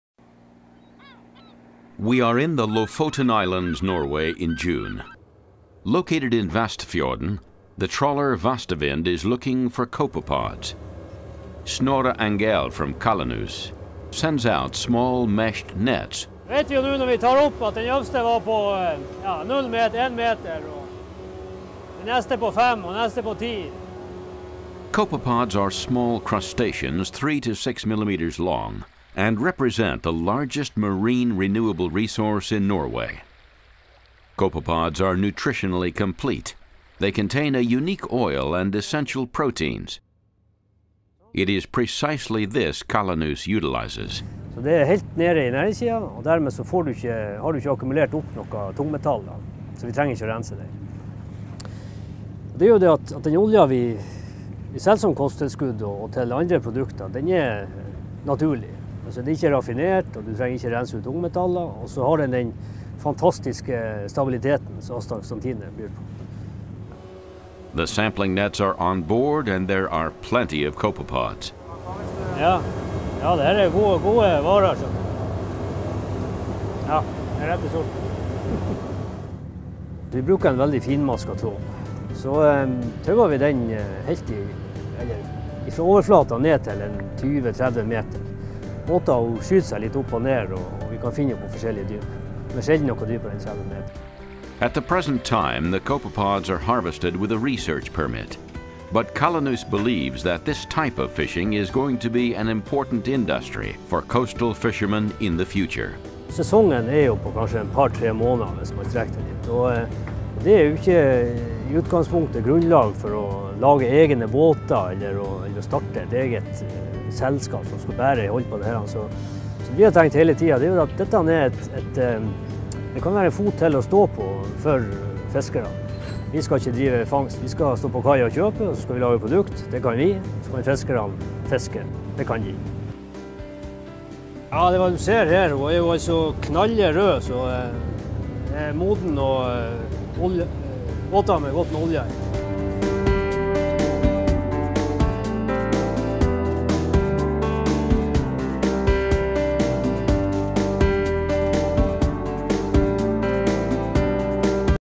Harvesting Arctic Ruby Oil from Calanus finmarchicus
There is a tiny zooplankton floating far off the coast of Norway. Calanus finmarchicus is chemically unique, different from every living creature in the sea. Once thought to be undigestible because of its wax monoesters, recent American studies have shown, the omega-3 fatty acids contained in the Arctic Ruby Oil extracted from Calanus are equal to if not better than the pharmaceutical grade fish oil (Lovaza). Further research confirmed, the Arctic Ruby Oil found in Calanus is capable of producing tremendous health benefits not seen in any other fish oil or krill oil.